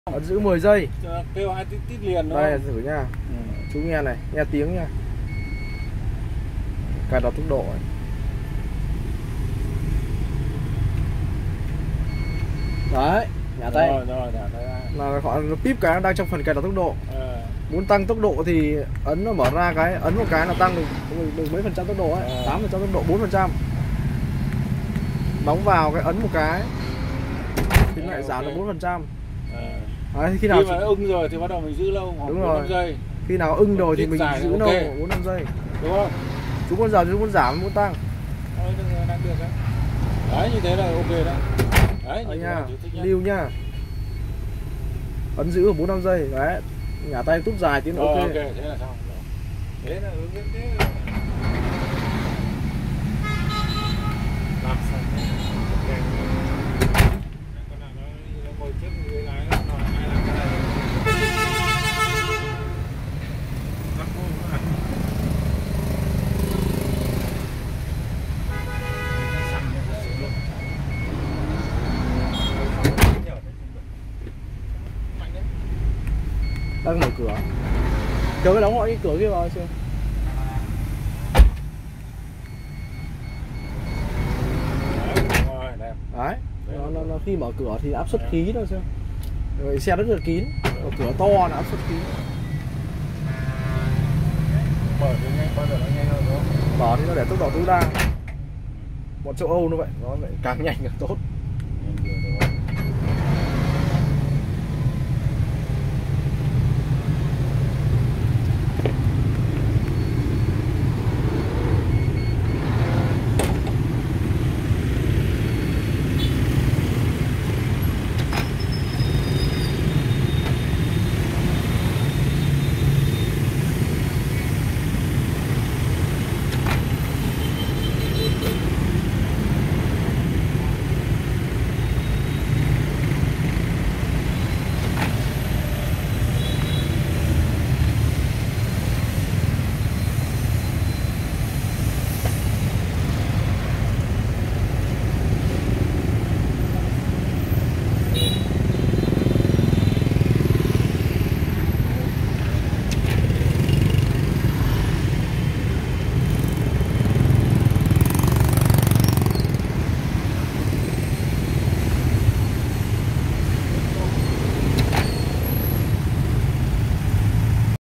Hướng dẫn cài đặt tốc độ cửa giấu kín cho xe Transit phiên bản Tpoko nhập khẩu Nga
Video hướng dẫn cài đặt tốc độ cho cửa tự động giấu kín, phiên bản Tpoko, nhập khẩu Nga, lắp đặt bởi Rambo Auto. Cửa tự động giấu kín Tpoko được thiết kế đặc biệt, giấu kín hầu hết các chi tiết của cơ cấu. Thoạt nhìn khó nhận biết đã lắp cửa tự động hay chưa.
Ưu điểm:
- Hệ thống kín
- Mạnh mẽ, đóng mở khoảng thời gian 3s
- Bền bỉ (sản phẩm chính hãng Adop- Nga)
- Mở đến gần cuối, hệ thống sử dụng thuật toán để phanh, do đó không có tiếng huých vào thân xe.